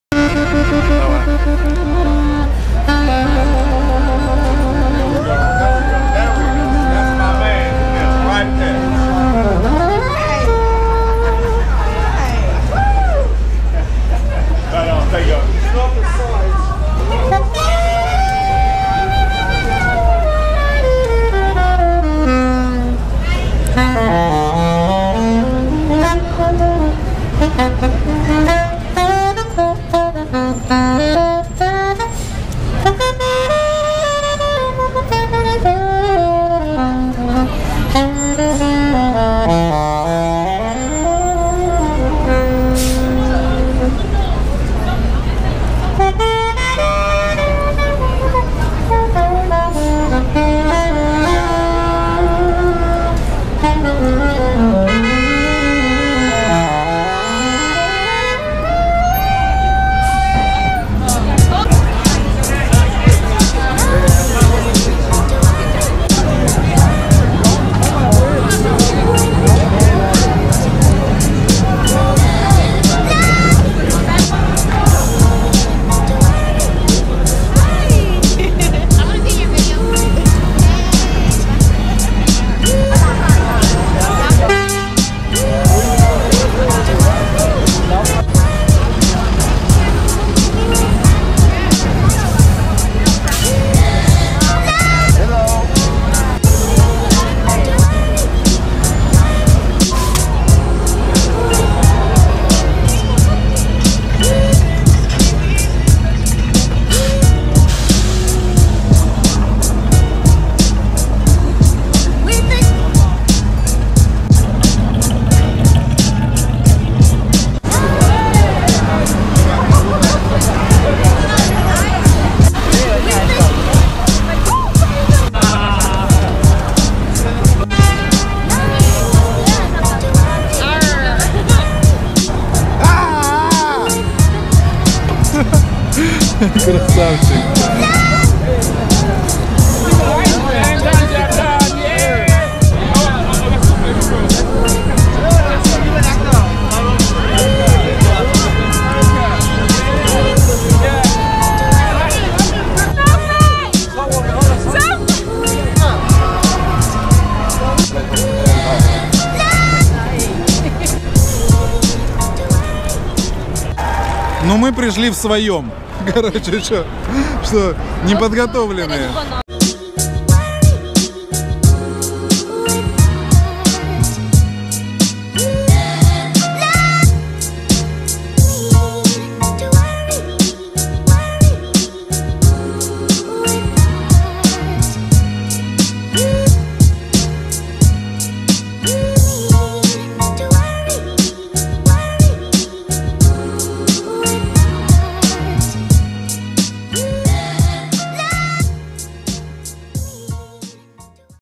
НЕ ВЛОГ: Halloween. New York.Без комментариев.
В этом влоге мы идем по Таймс Сквер в Нью Йорке на Хэллоуин .(Halloween). Я очень хотел передать атмосферу улицы , поэтому не стал ничего комментировать.